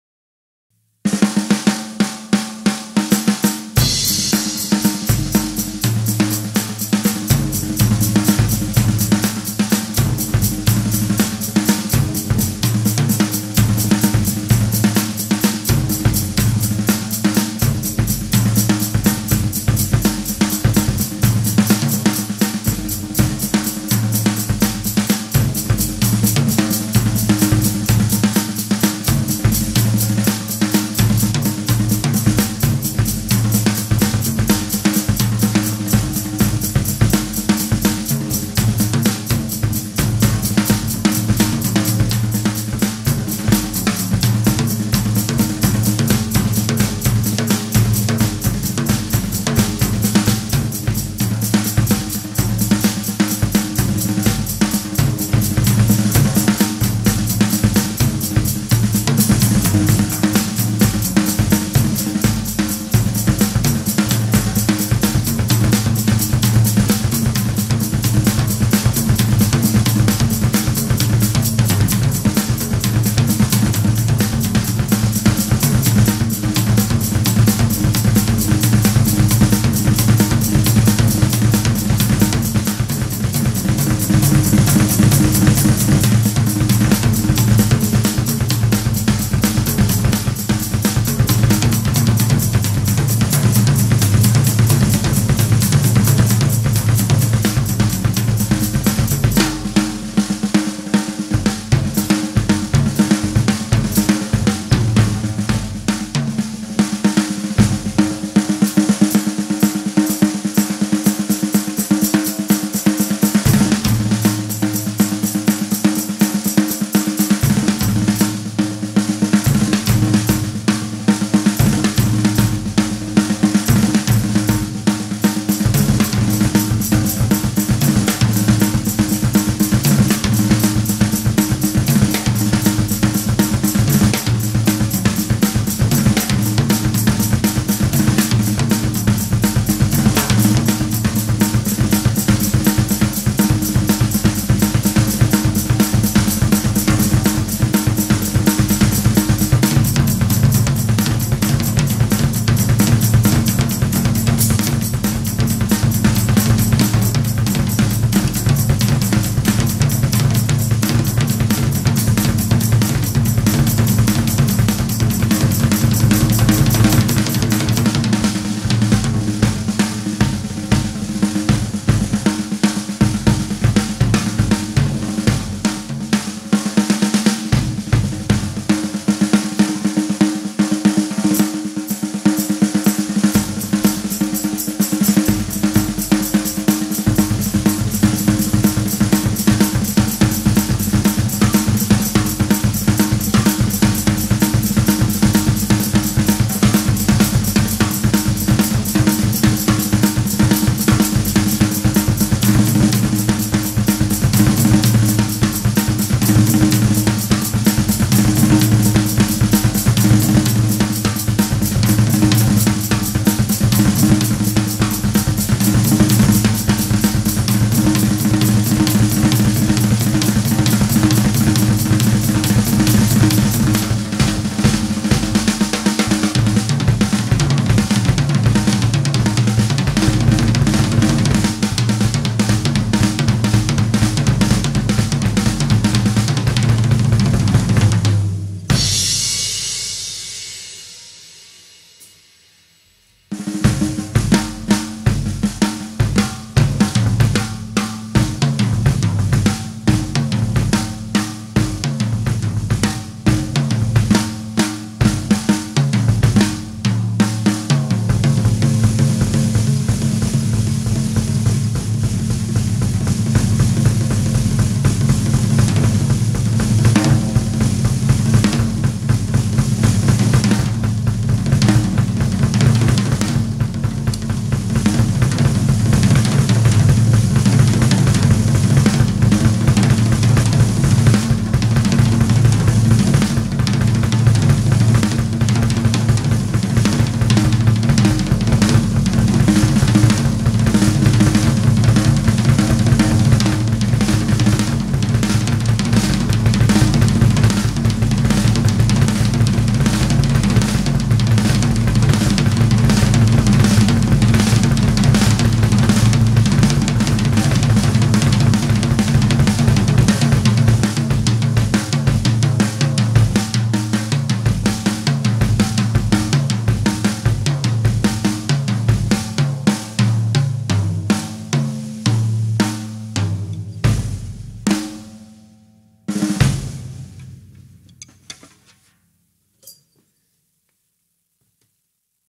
Vistalite Solo for Tuning Video
I have all the footage shot for the tuning video! Now I just need to edit it all together. As a teaser, here is a clip of what the kit sounds like. This will be included in the video, along with other samples. Happy New Year everyone!